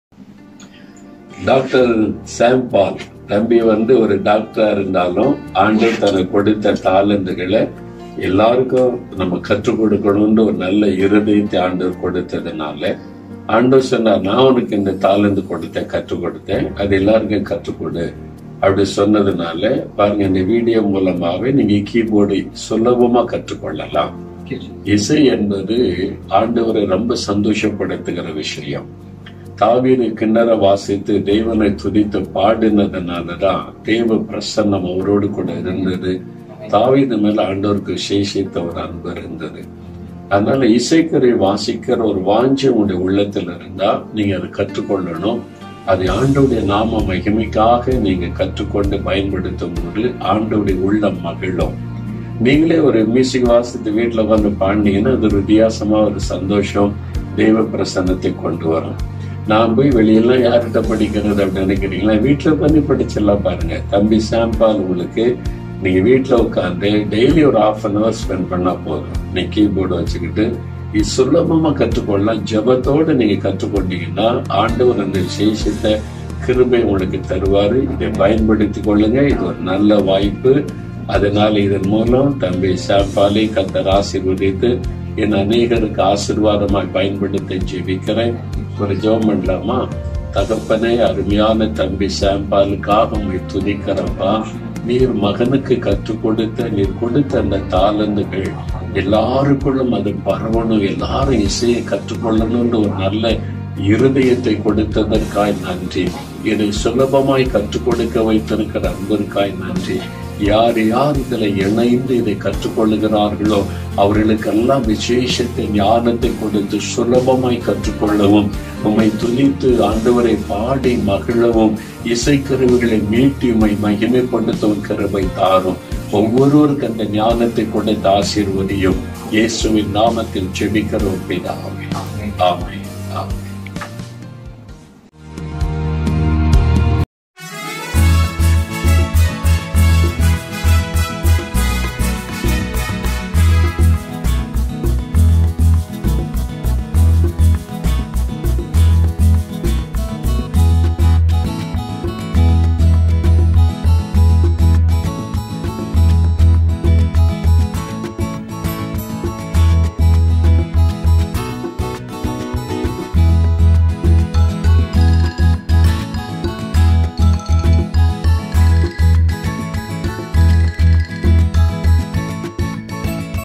Brother Mohan C Lazarus about Gospel Keyboard Tutorial | Jesus Redeems | Special Video
This video is so special to me ❤️

Brother Mohan C Lazarus, a person who used to bring the revival across the Nations, made a special video for our channel. His loving words and prayers brings more motivation to our channel and blessings indeed.

MAKE MUSIC EASILY is a special series in which we are going to meet many Pastors and Brothers to know about their music life.